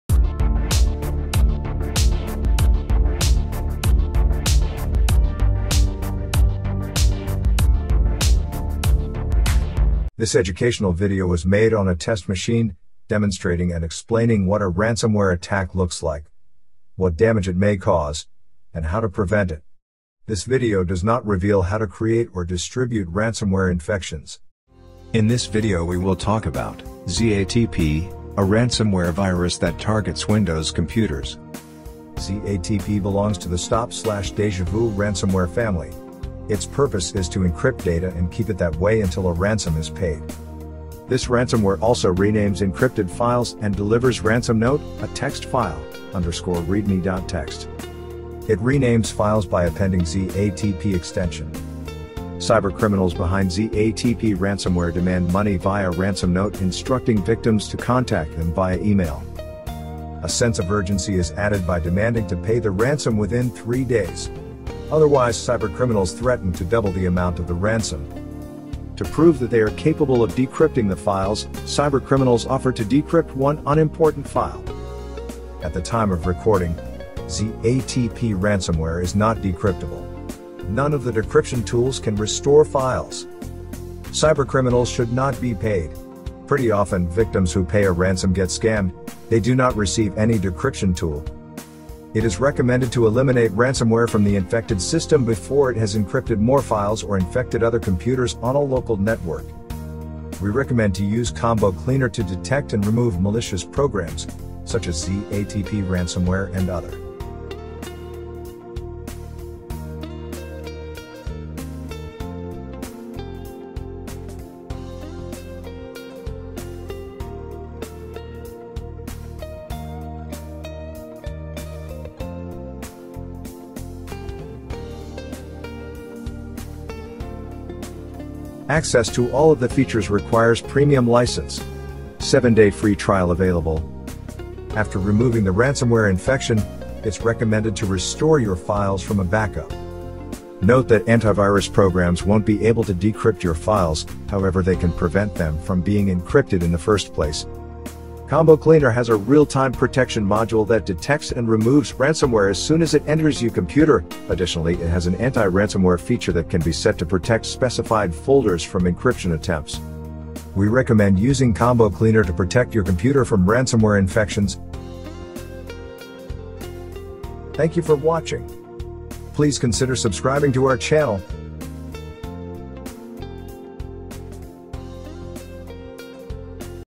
Zatp ransomware (.zatp virus). Removal instructions.
Video showing what to do in a case of Zatp ransomware.